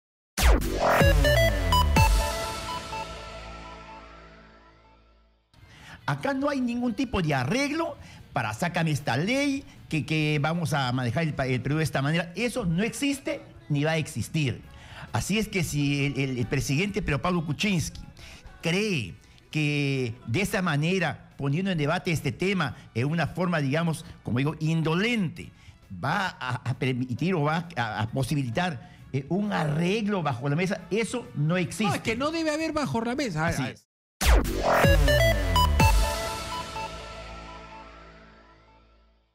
Phillip Butters y Congresista Héctor Becerril sobre indulto de Fujimori
“No habrá arreglo político poniendo como condición la libertad de Fujimori”
El congresista de Fuerza Popular instó a PPK a no buscar un "acuerdo bajo la mesa" usando la libertad de Alberto Fujimori.